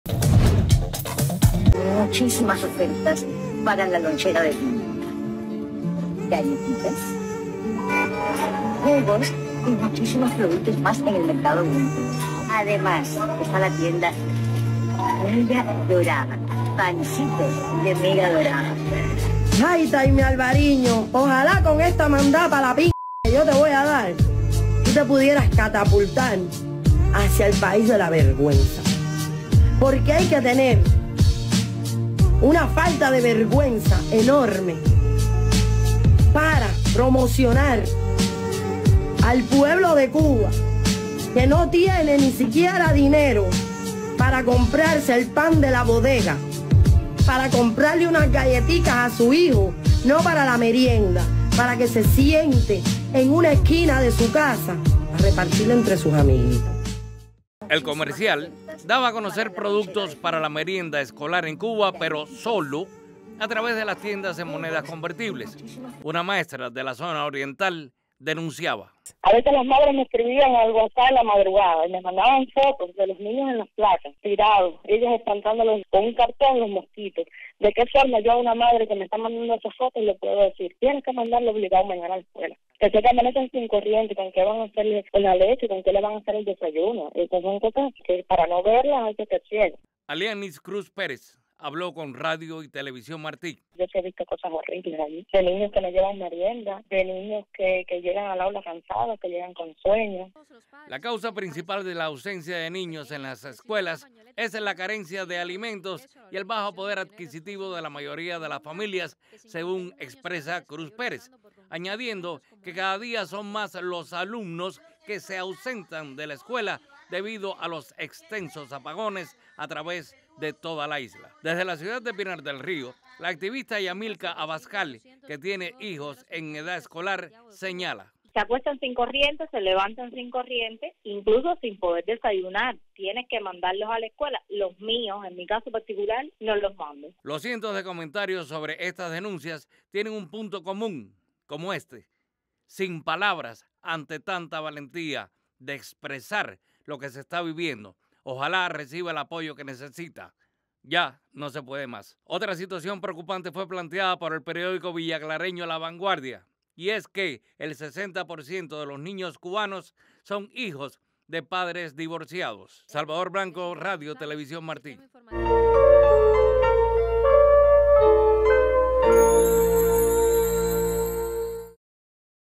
Info Martí | La asistencia escolar en Cuba marcada por la crisis imperante en la Isla
Las dificultades de las madres para llevar a sus hijos a la escuela en Cuba aumentan en medio de la escasez, los apagones  y la falta de recursos.